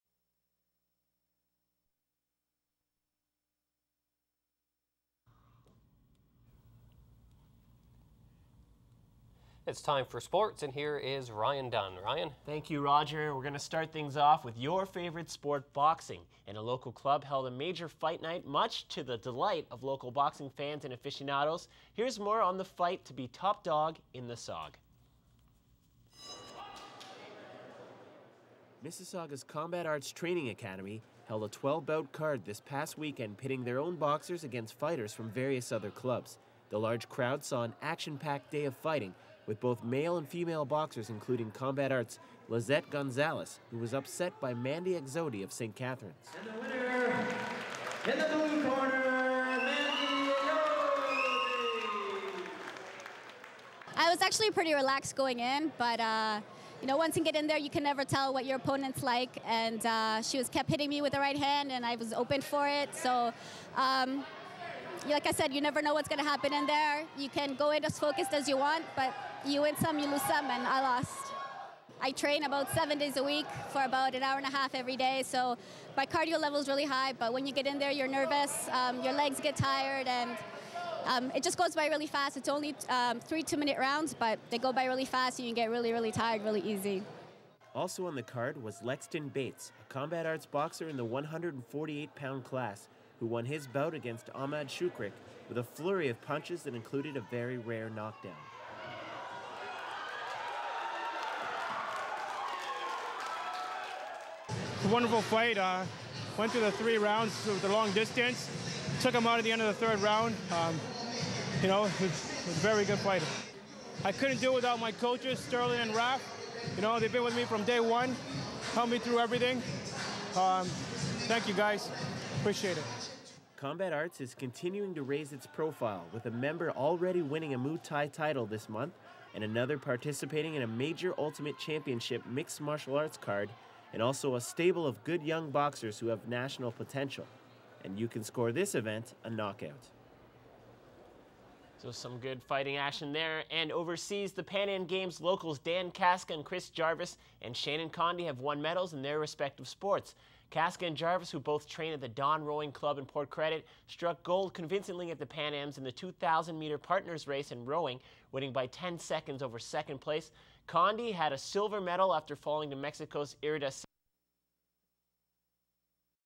Boxing
Lexton Bates knocking out Jamestown Fighter